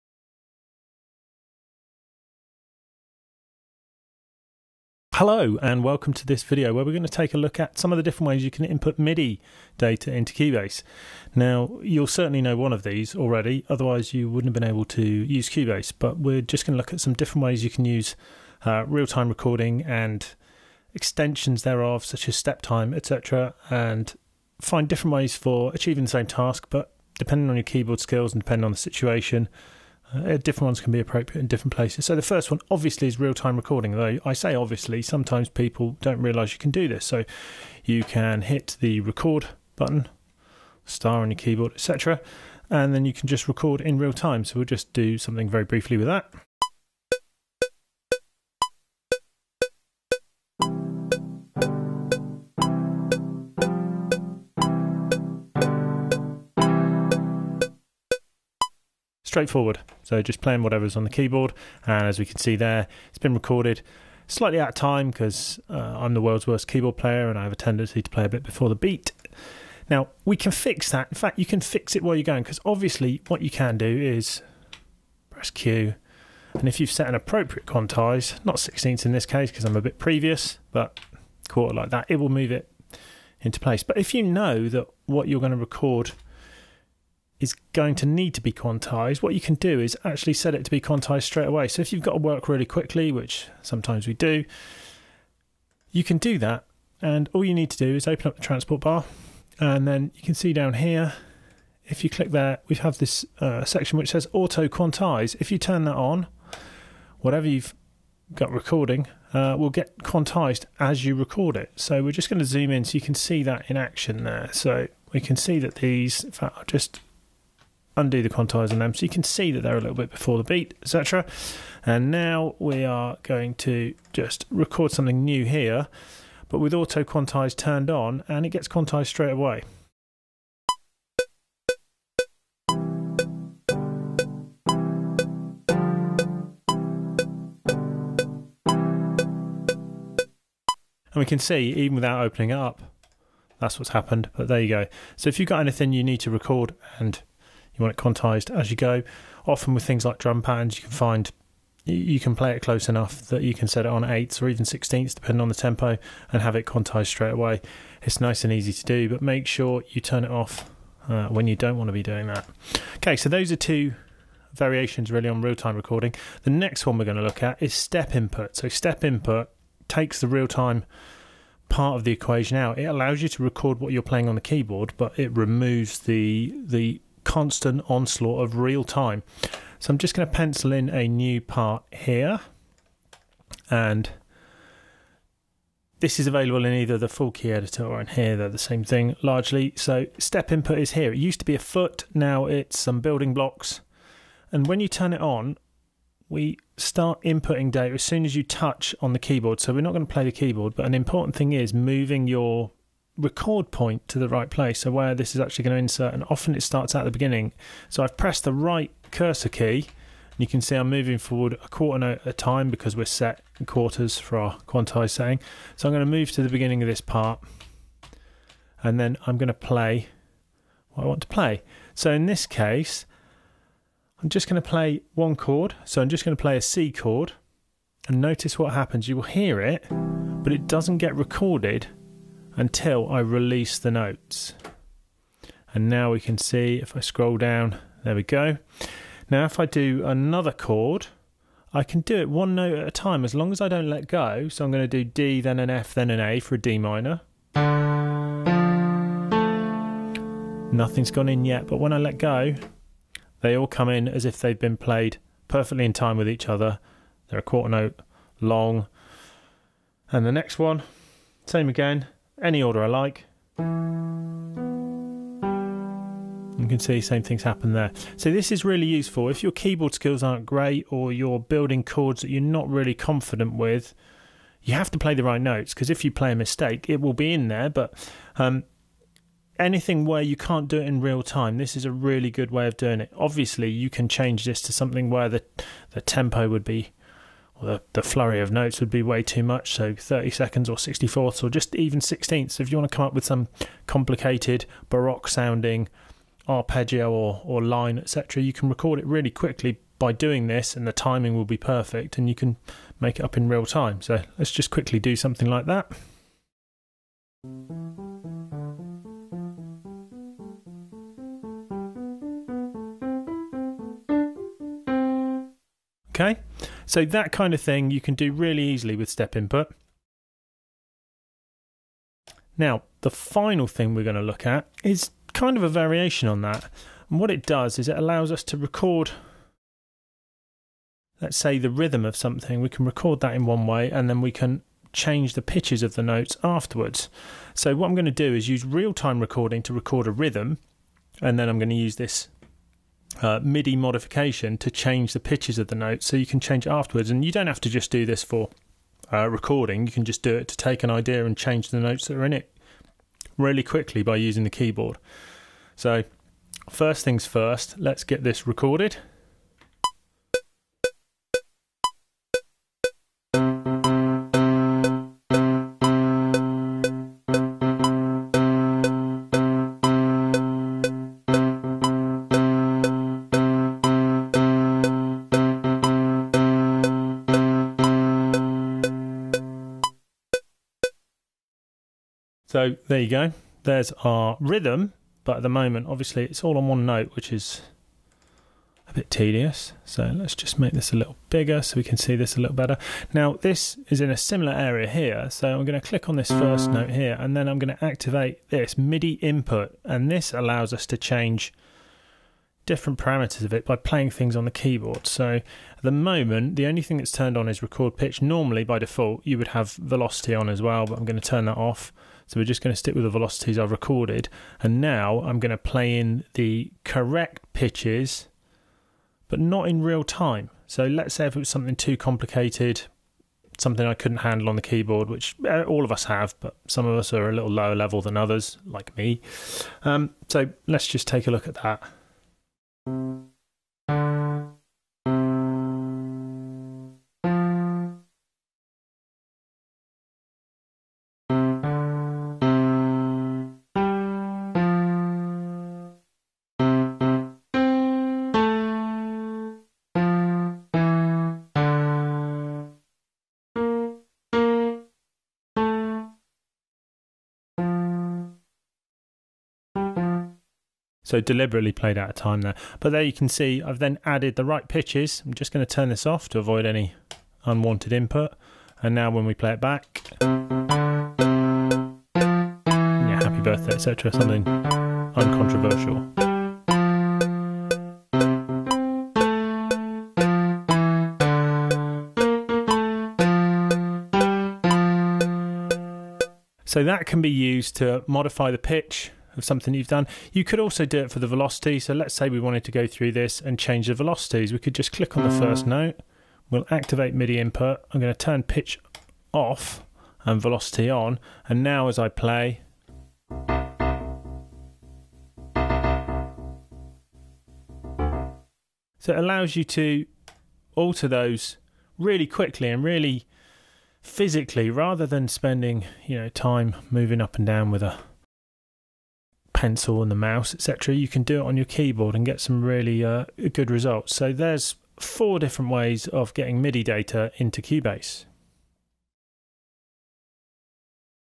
Cubase Skills: MIDI Input Methods
A look at different ways to enter MIDI data from the keyboard, to allow creation of complex parts which are not playable in real time, as well as alteration of pitch or velocity of an existing part.